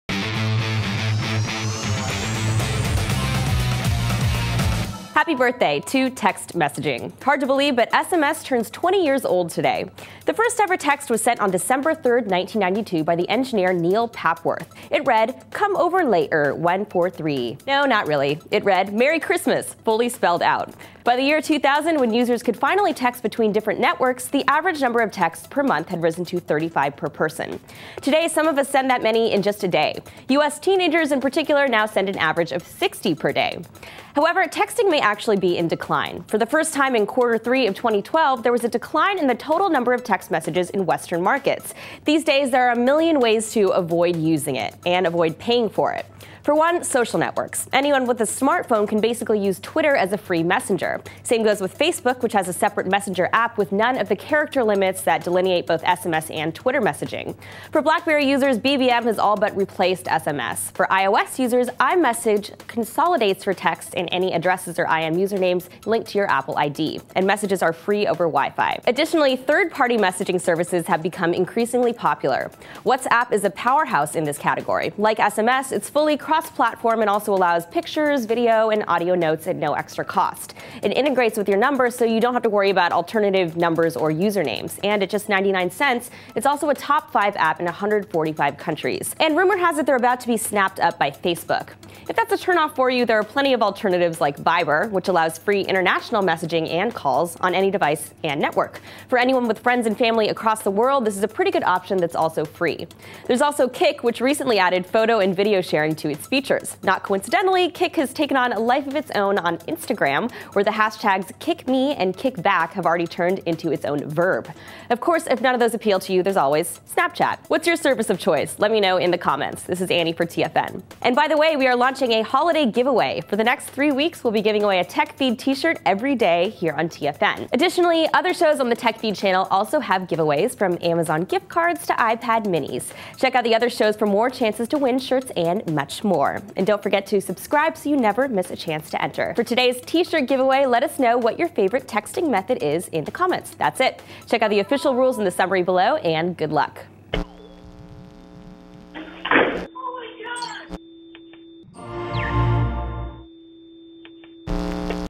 Happy 20th Birthday Text Messaging :)
Today is the 20th anniversary of the first text message ever sent. Annie takes a look back at the history of sms messaging, and examines why it may be declining. She also announces the details of Tech Feed's Holiday Giveaway- make sure to tune in every day for your chance to win!